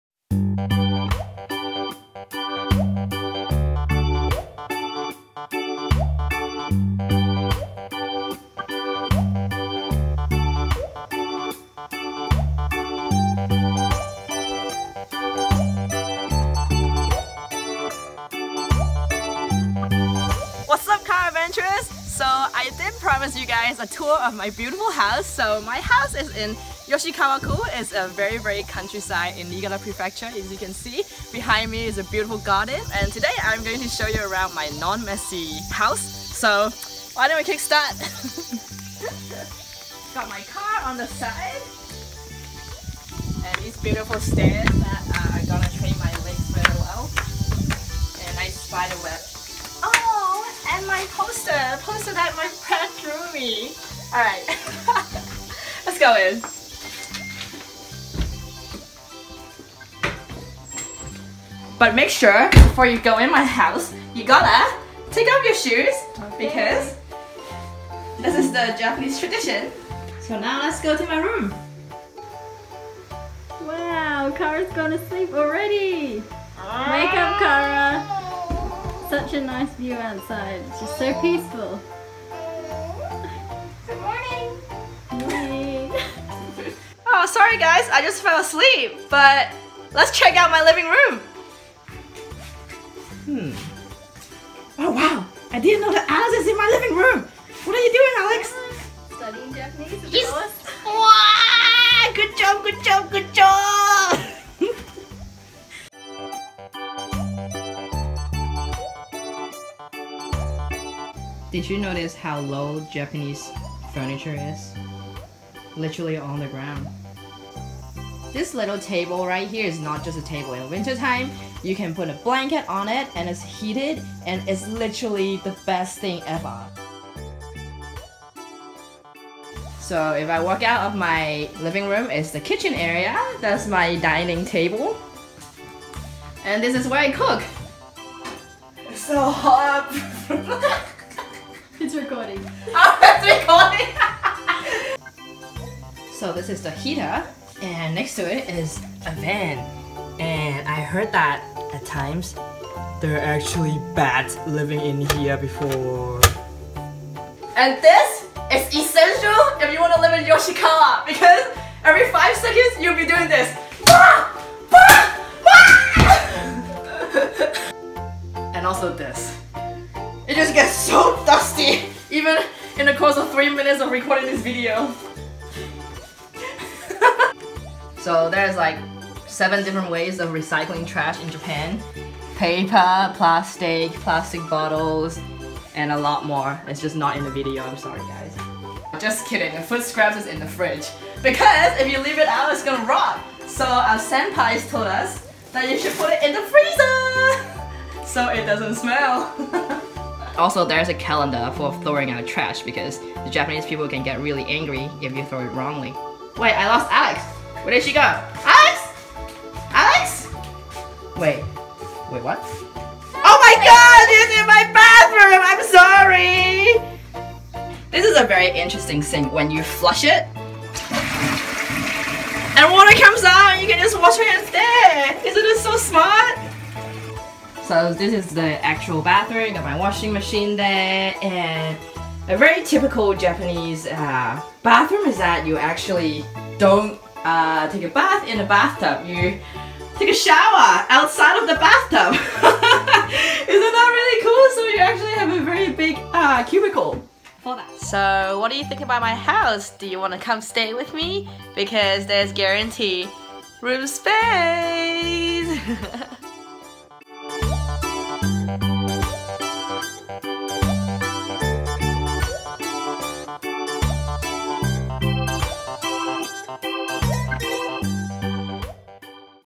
JET Program: My 33,000yen/month HOUSE in Rural Japan!
For ONCE and ONCE ONLY, I actually cleaned up my house to showcase what a countryside Japanese home @ Niigata looks like! Ft. cicada whistles, tatami mats, Japanese bathtubs, my beautiful neighbor, and giggles.

PS. Insta is cool too, follow me on TheCaradventures! :)